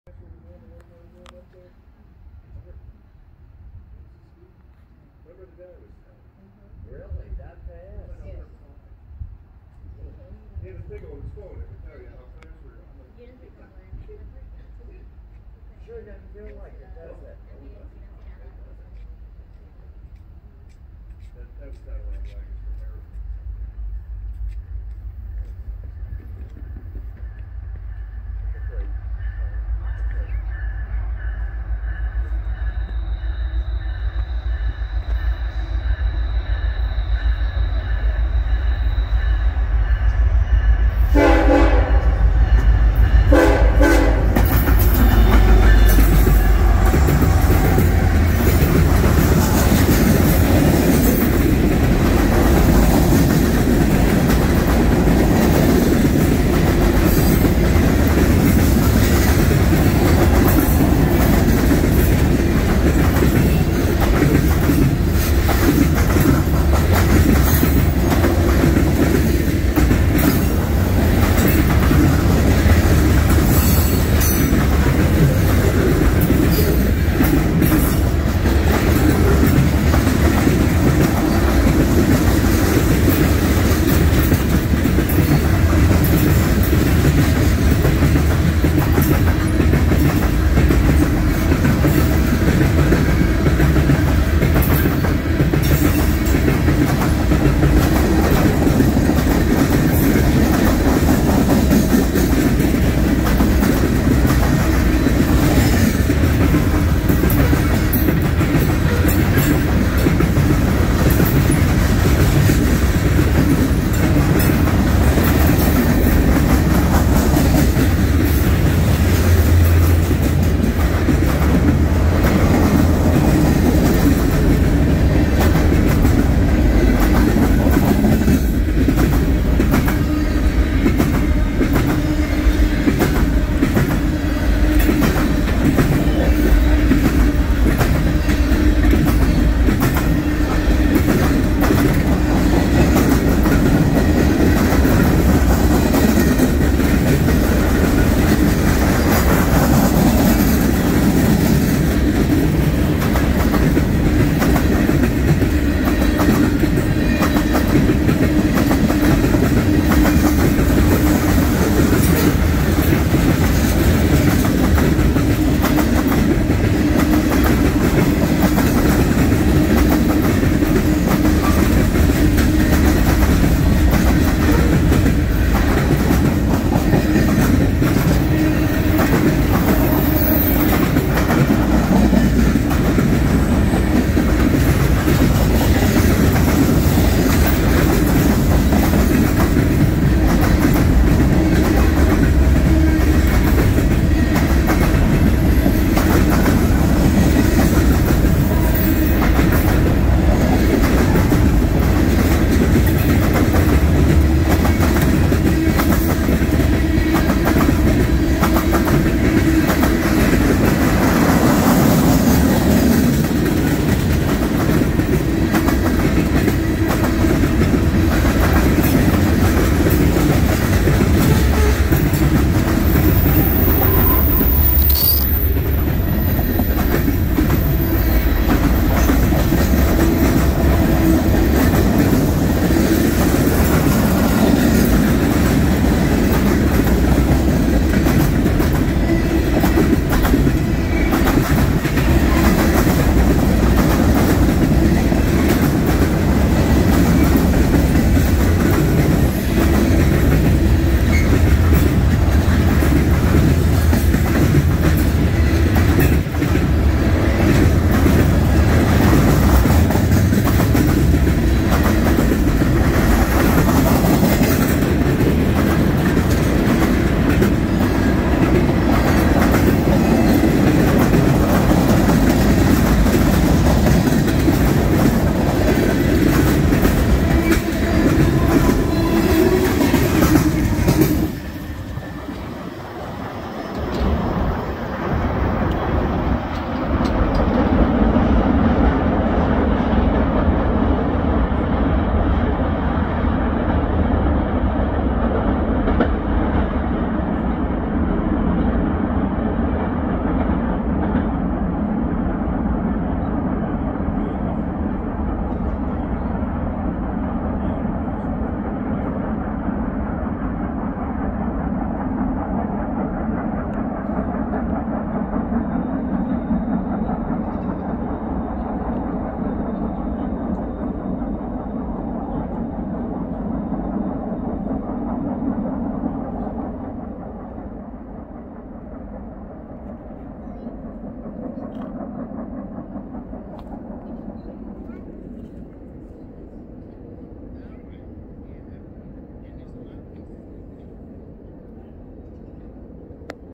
Norfolk Southern 1803 leads a 170 car manifest at Lewistown, PA (12/26/2021)
13V -